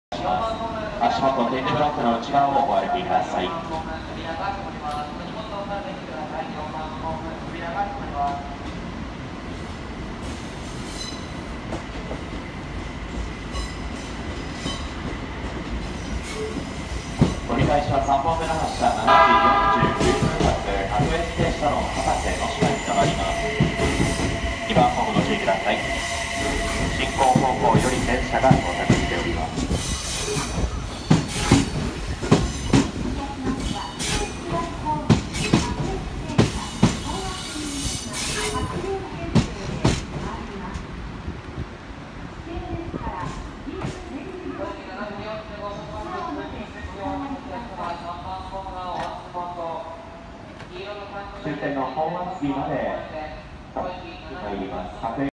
ポイント切り替えが早すぎる！【過密ダイヤ】
昨日もアップした小田急の逆走動画。それが通るポイントは切り替わるのが京急並みに早いです！